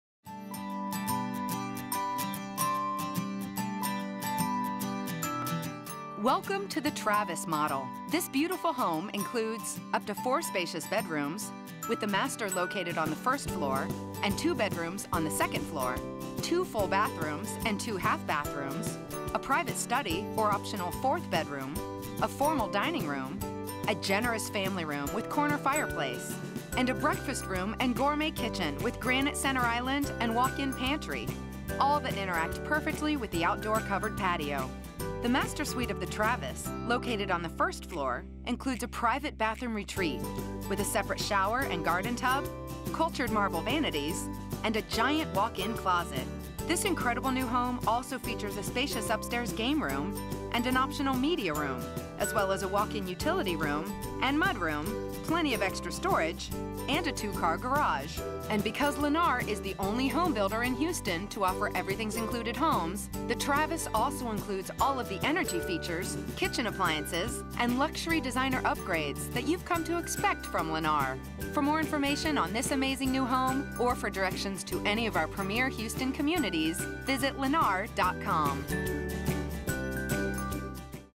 The Travis New Home Tour - Lennar Houston
Welcome to The Travis model, This beautiful home includes; up to four spacious bedrooms, with the master located on the first floor, and 2 bedrooms on the second floor, two full bathrooms and two half bathrooms, a private study or optional fourth bedroom, a formal dining room, a generous family room with corner fireplace, and a breakfast room and gourmet kitchen with granite center island and walk in pantry; all that interact perfectly with the outdoor covered patio. The master suite of the Travis, located on the first floor, includes a private bathroom retreat with a separate shower and garden tub, cultured marble vanities and a giant walk-in closet. This incredible new home also features a spacious upstairs game room and an optional media room, as well as a walk-in utility room and mudroom plenty of extra storage and a 2-car garage. And because Lennar is the only home builder in Houston to offer Everything's Included homes, the Travis also includes all of the energy features, kitchen appliances and luxury designer upgrades that you have come to expect from Lennar. For more information on this amazing new home or for directions to any of our Premier Houston Communities